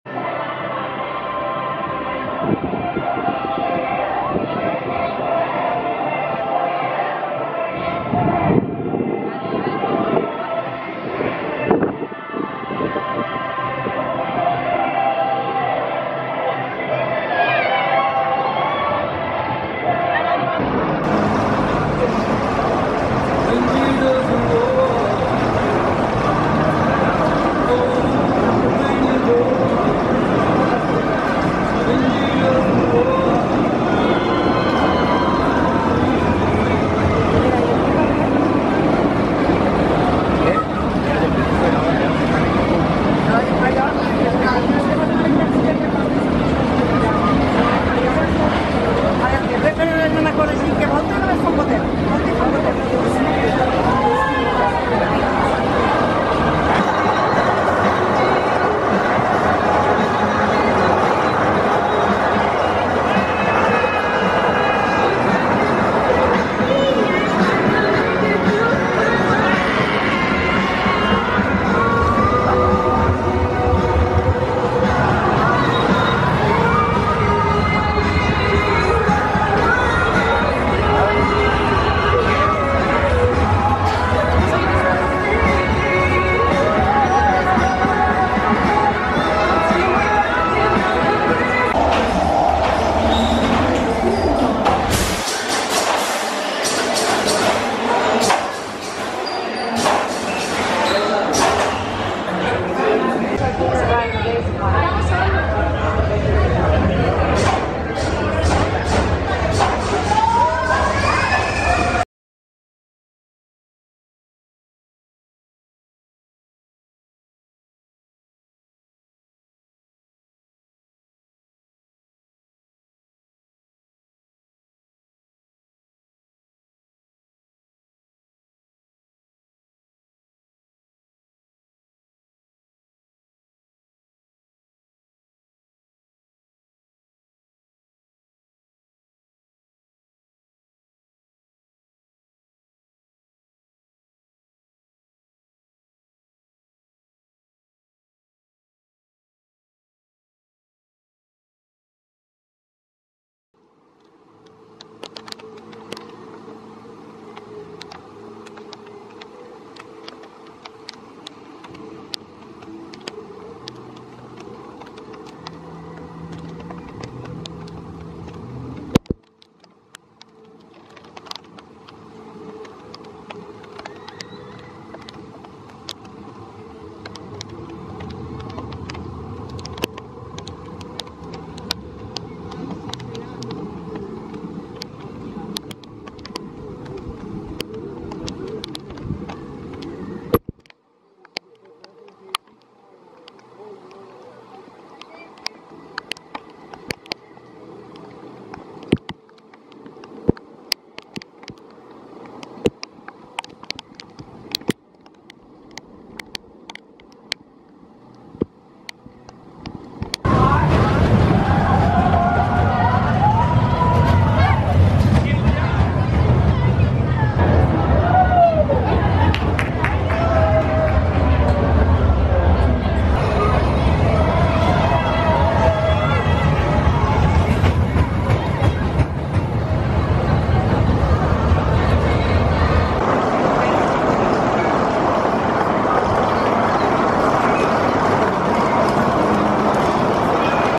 El Diablo- Tren de la Mina Off-Ride Footage, PortAventura Arrow Mine Train | Non-Copyright
El Diablo is the Arrow mine train at PortAventura and it is the Spanish resort's best family coaster.  This Mexican themed coaster travels over rides and through tunnels and pits.